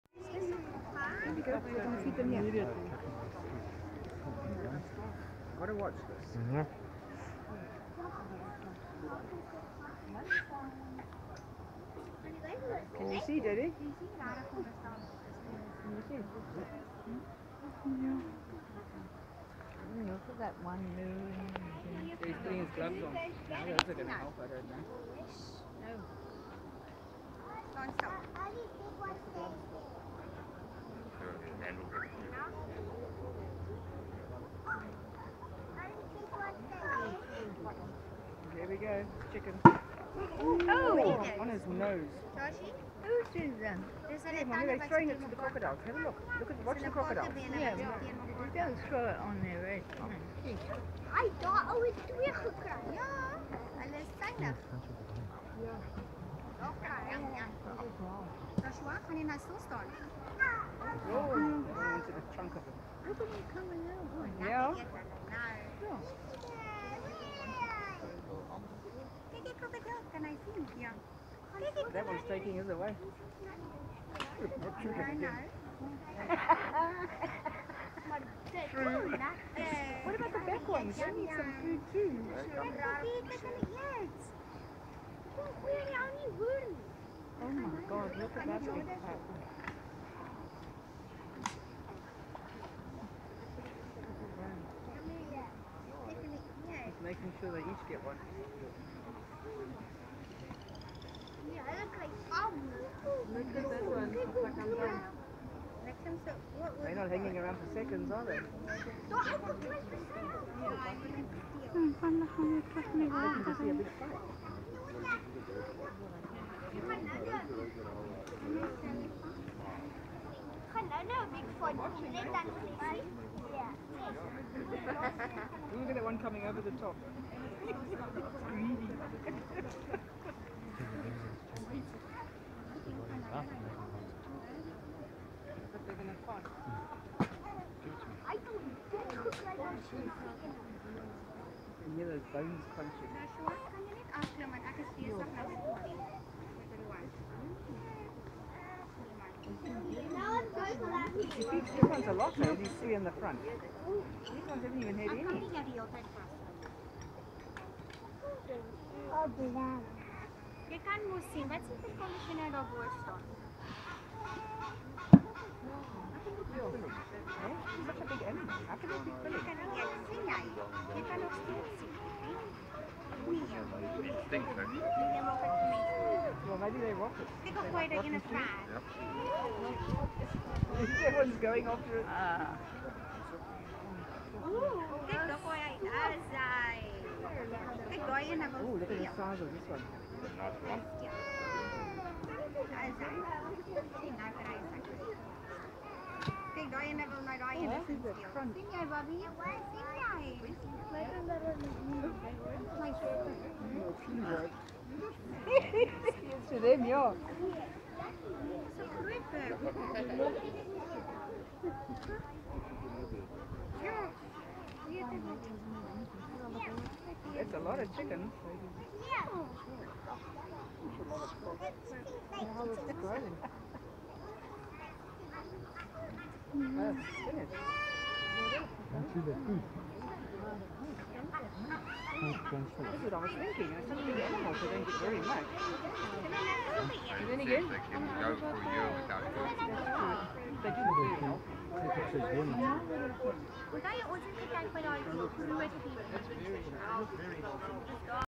Who let the Croc's Out? Amazing bite force! (Part 2)
We had a great time at CrocWorld, where they showed us how they feed their crocodiles and also shared some fun facts about crocodiles and in which habitat they live and how often they need to be fed.

As our friends move closer with the meat to feed the crocodiles, you can see more and more coming out of the water to enjoy their lunch.